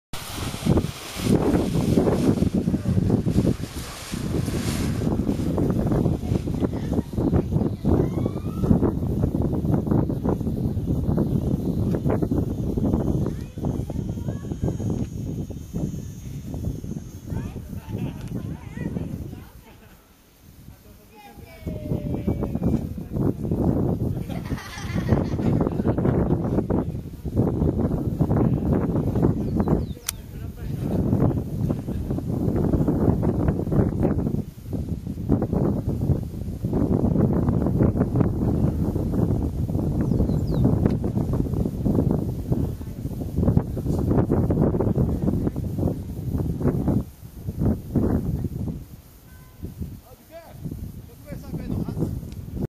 Israel Nho decolando da rampa de Parapente de Santa Rita do Sapucaí
Nho decolando com sua vela nova na rampa de Santa Rita do Sapucaí.